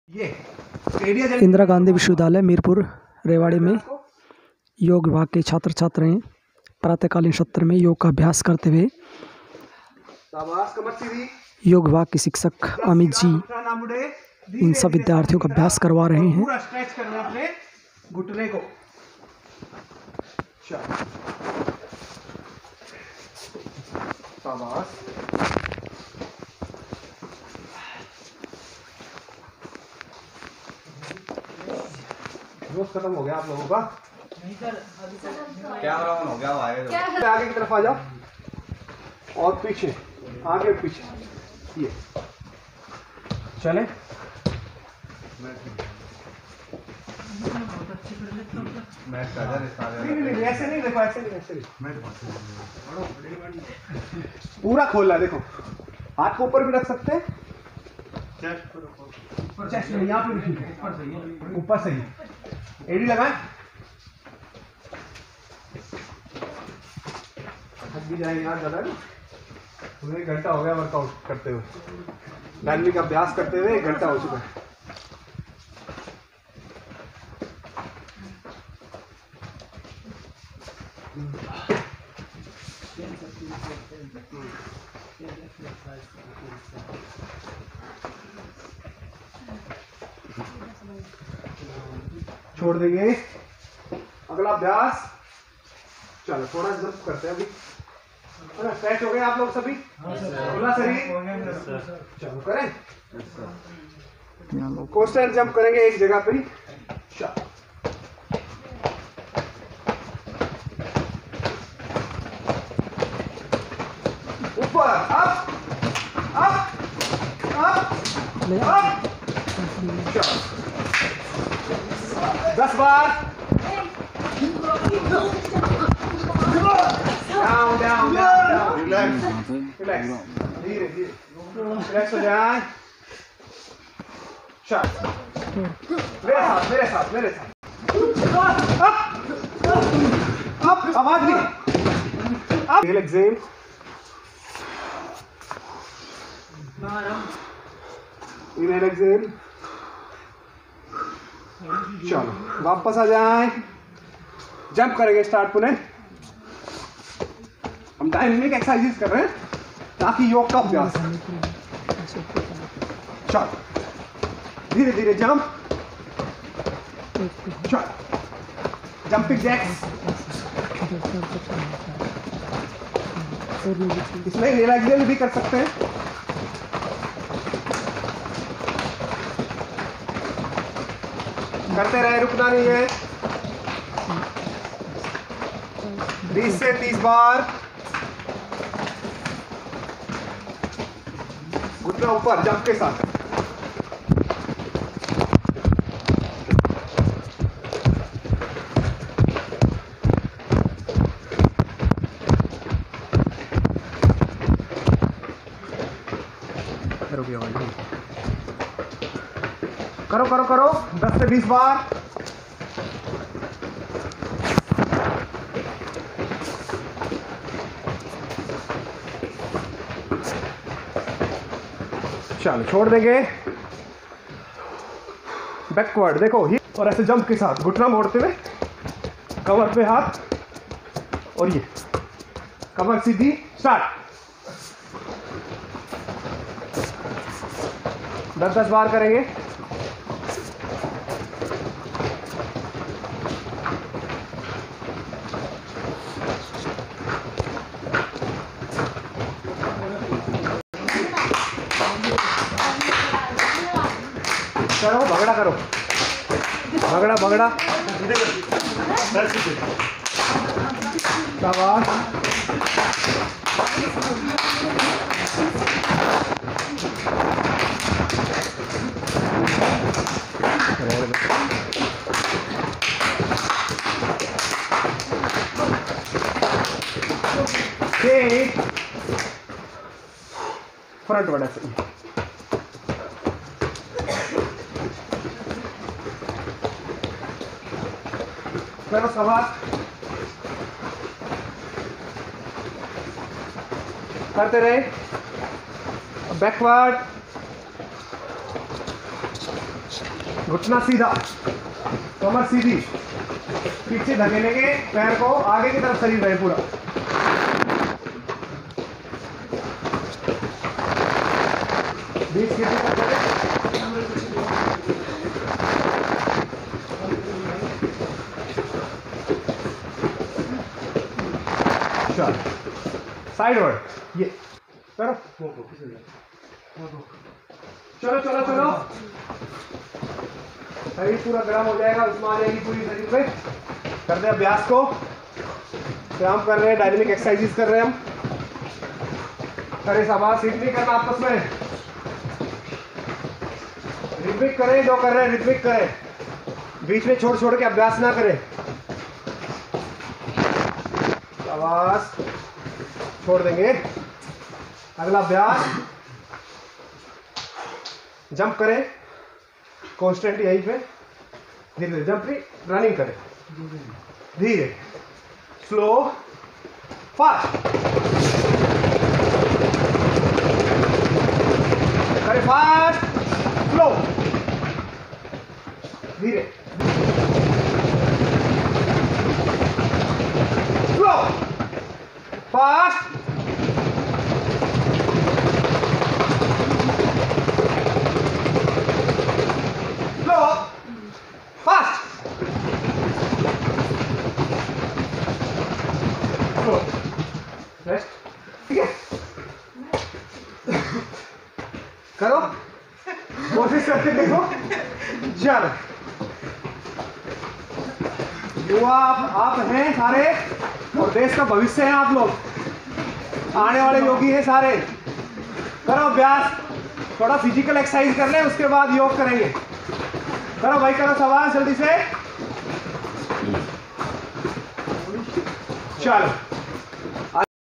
Yogic Warm-up#How to warm up in winter season?सर्दियों के मौसम में कैसे करें वार्म अप?
Yogic jogging#Common warmup exercise in winter season
powerful warmup exercise for all
Yoga instructor Amit sir
Dr.Dharamveer Yog acharya